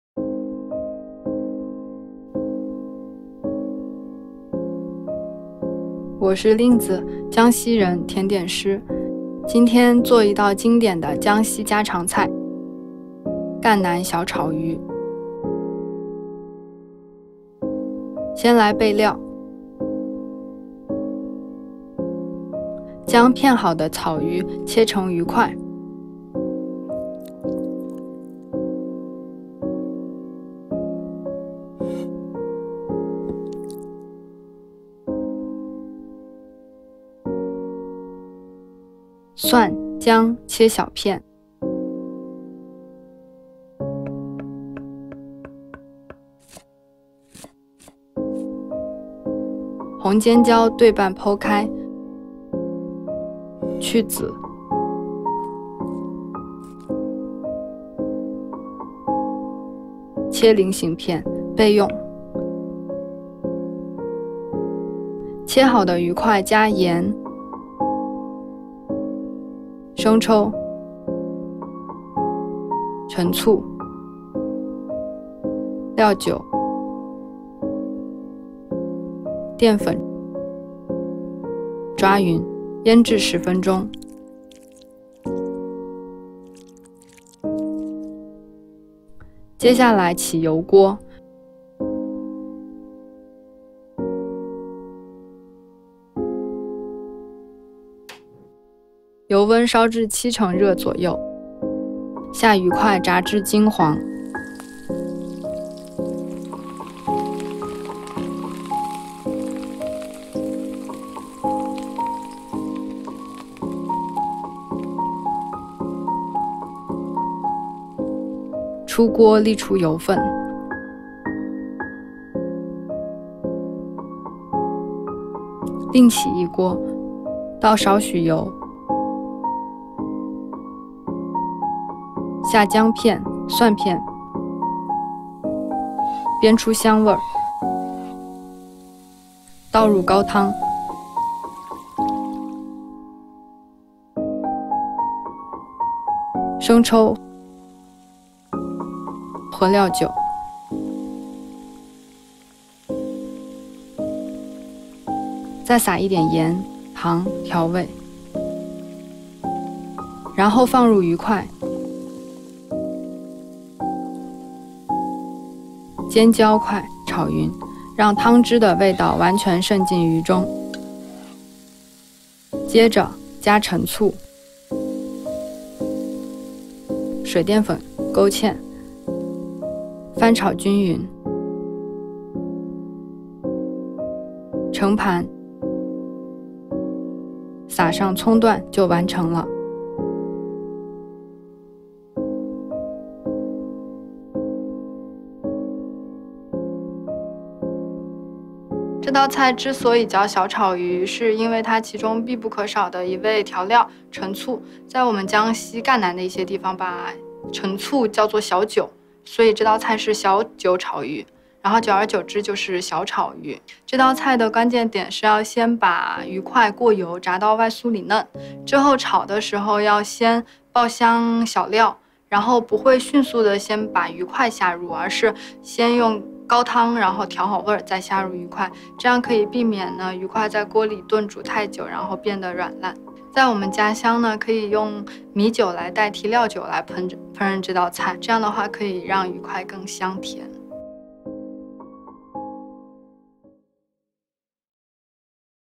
美食台｜赣南小炒鱼
草鱼是很多人的餐桌常客，赣南人做草鱼很有一套，鱼块先过一次油再炒，香味浓郁，外酥里嫩。而醋用的到不到位，也是这道菜好不好吃的关键。当地人管它叫“小炒鱼”，实则是小酒炒鱼，所谓“酒”，其实就是醋。